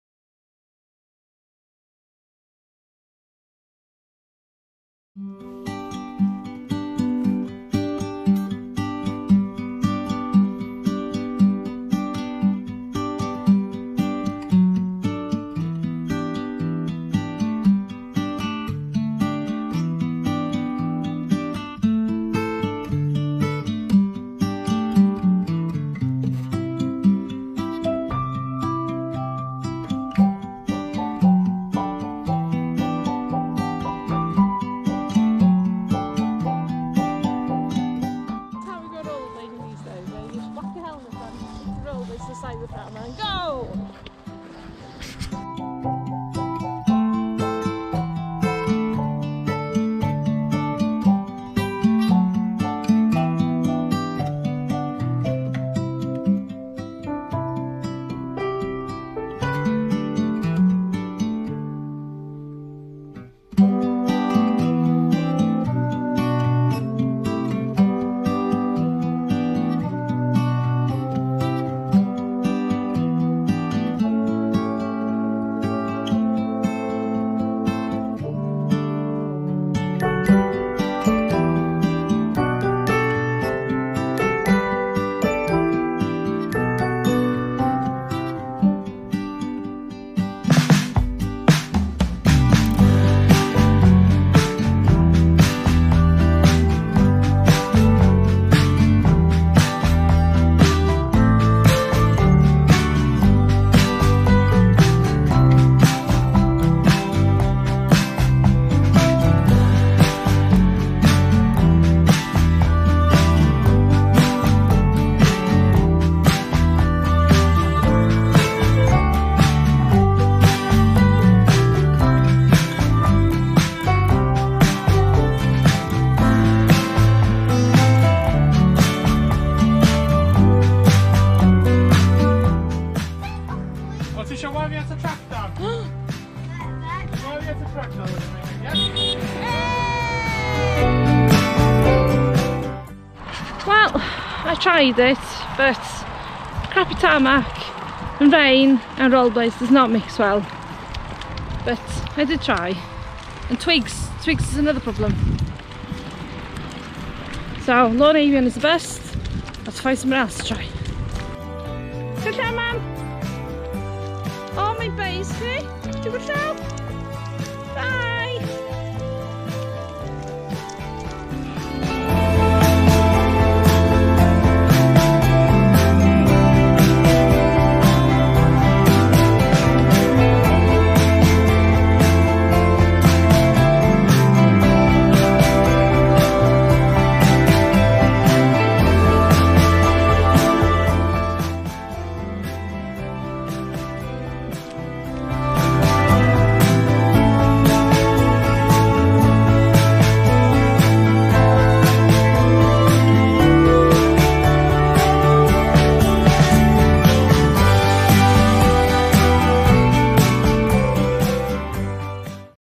Lon Eifion - Inigo Jones to Penygroes
Well my plan to rollerblade along Lon Eifion was an epic fail! Too slippery, too many twigs to navigate and bumpy tarmac is not conducive to good rollerblading!

So instead we walked it. Stood at the bridge in Penygroes waving at cars! :)
Footage filmed on GoPro Hero 7
Edited in Quik & Splice